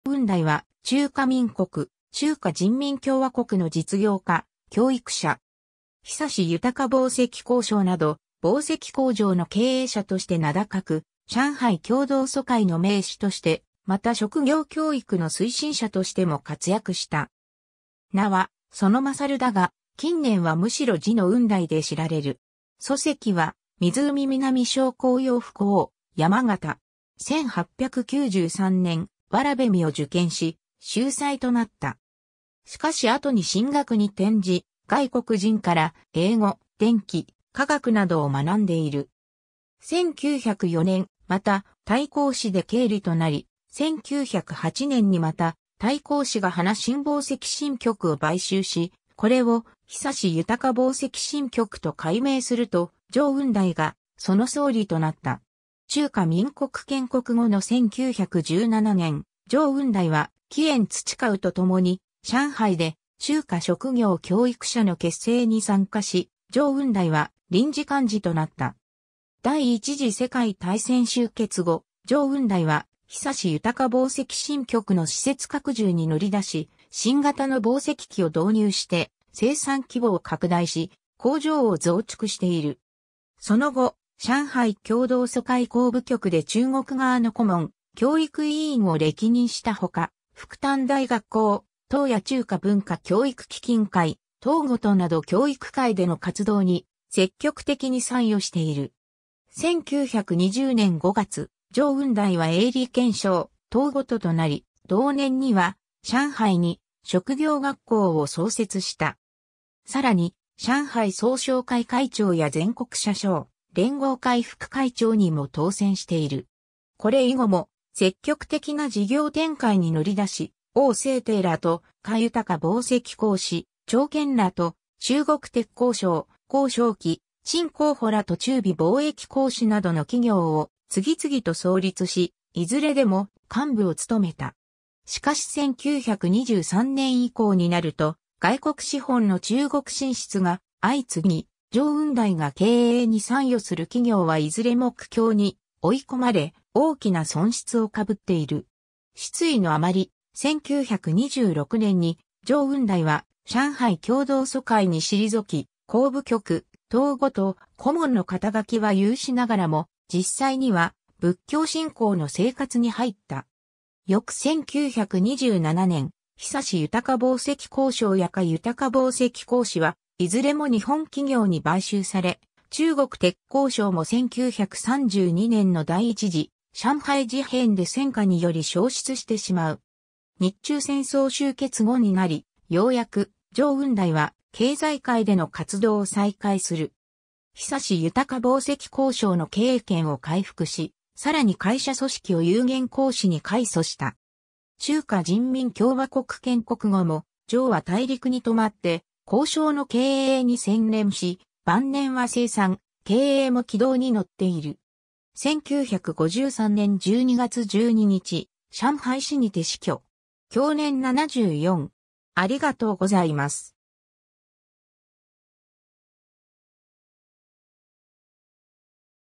聶雲台
聶 雲台（じょう うんだい）は、中華民国・中華人民共和国の実業家・教育者。
恒豊紡績工廠など紡績工場の経営者として名高く、上海共同租界の名士として、また職業教育の推進者としても活躍した。
名は其杰だが、近年はむしろ字の雲台で知られる。
祖籍は湖南省衡陽府衡山県。
 1893年（光緒19年）、童試を受験し、秀才となった。
しかし後に新学に転じ、外国人から英語・電気・化学などを学んでいる。
1904年（光緒30年）、復泰公司で経理となり、1908年（光緒34年）に復泰公司が華新紡績新局を買収し、これを恒豊紡績新局と改名すると、聶雲台がその総理となった。
 中華民国建国後の1917年（民国6年）、聶雲台は黄炎培と共に上海で中華職業教育社の結成に参加し、聶雲台は臨時幹事となった。
第一次世界大戦終結後、聶雲台は恒豊紡績新局の施設拡充に乗り出し、新型の紡績機を導入して生産規模を拡大し、工場を増築している。
その後、上海共同租界工部局で中国側の顧問・教育委員を歴任した他、復旦大学校董や中華文化教育基金会董事など教育界での活動に積極的に参与している。
 1920年（民国9年）5月、聶雲台は永利鹸廠董事となり、同年には上海に職業学校を創設した。
さらに、上海総商会会長や全国紗廠聯合会副会長にも当選している。
これ以後も積極的な事業展開に乗り出し、王正廷らと華豊紡績公司、張謇らと中国鉄鋼廠、孔祥熙・陳光甫らと中美貿易公司などの企業を次々と創立し、いずれでも幹部を務めた。
 しかし1923年（民国12年）以降になると、外国資本の中国進出が相次ぎ、聶雲台が経営に参与する企業はいずれも苦境に追い込まれ、大きな損失を被っている。
失意の余り、1926年（民国15年）に聶雲台は上海共同租界に退き、工部局董事・顧問の肩書きは有しながらも、実際には仏教信仰の生活に入った。
翌1927年（民国16年）、恒豊紡績工廠や華豊紡績公司はいずれも日本企業に買収され、中国鉄鋼廠も1932年（民国21年）の第一次上海事変で戦火により消失してしまう。
 日中戦争終結後になり、ようやく聶雲台は経済界での活動を再開する。
恒豊紡績工廠の経営権を回復し、さらに会社組織を有限公司（有限会社）に改組した。
中華人民共和国建国後も聶は大陸に留まって工廠の経営に専念し、晩年は生産・経営も軌道に乗っている。
 1953年12月12日、上海市にて死去。